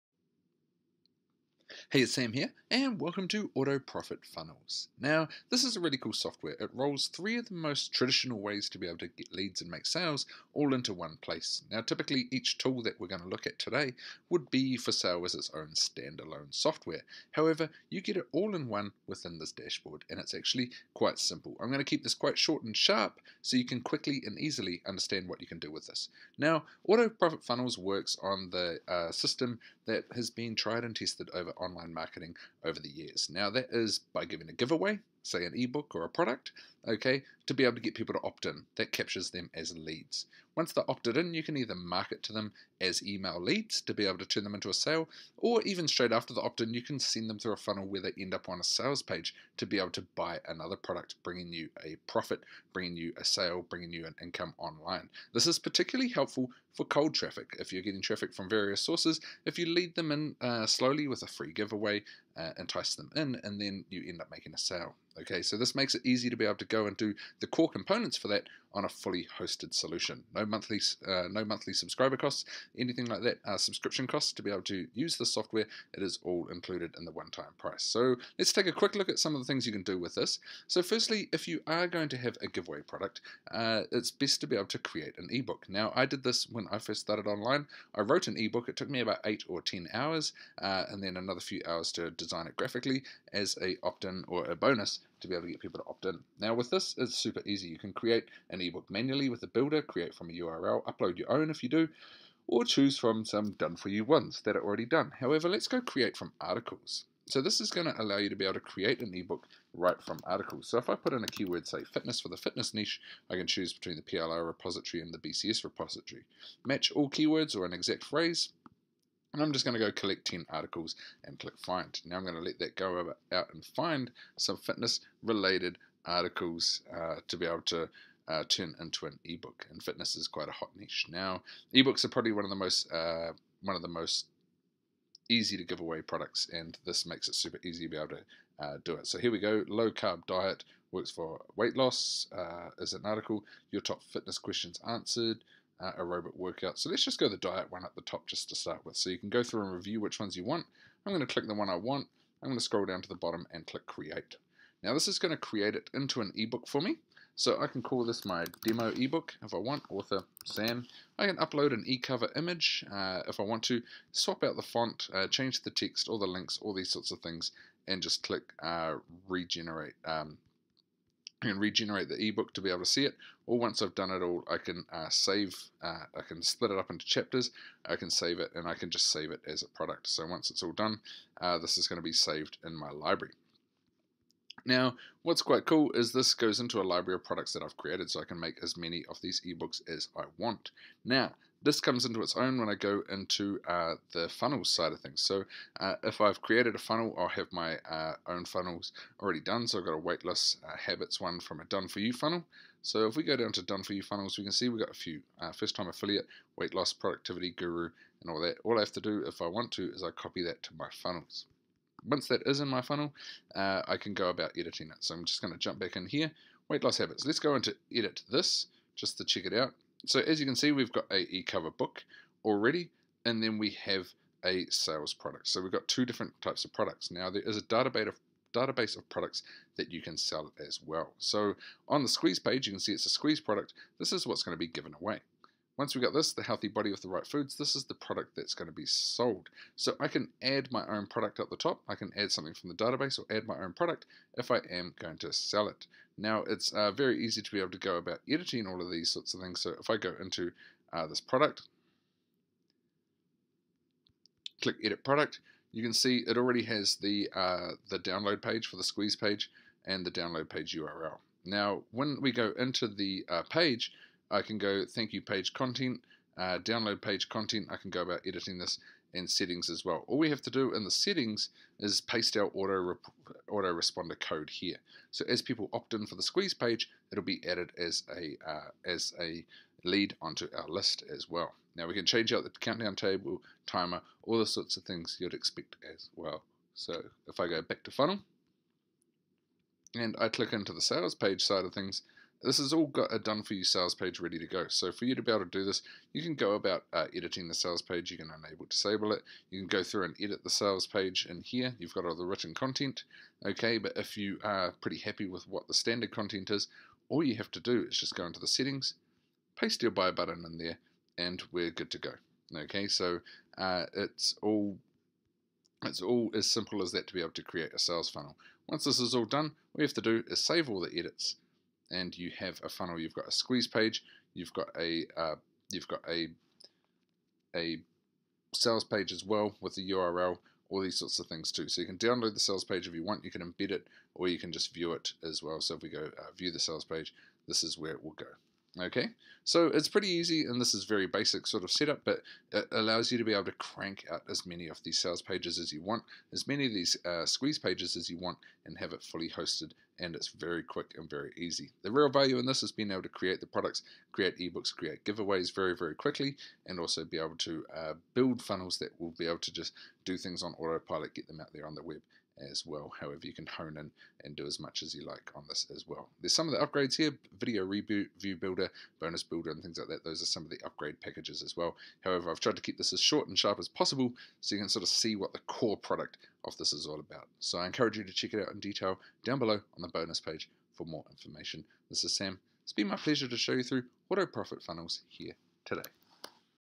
AutoProfit Funnels Demo
New Breakthrough Software Creates High Converting Squeeze Pages, Products & Sales Funnels With Just A Few Clicks Of Your Mouse...

- 100% Newbie Friendly Cloud Based Web App

- Never Before Seen Technology Creates Profitable Digital Product Funnels

- Chose From Library of Done-For-You Products

- Use The App To Create Your Very Own Digital Product

- Access To An Exclusive Library of Hundreds of Digital Products

- Nothing To Download - Hosted Securely Online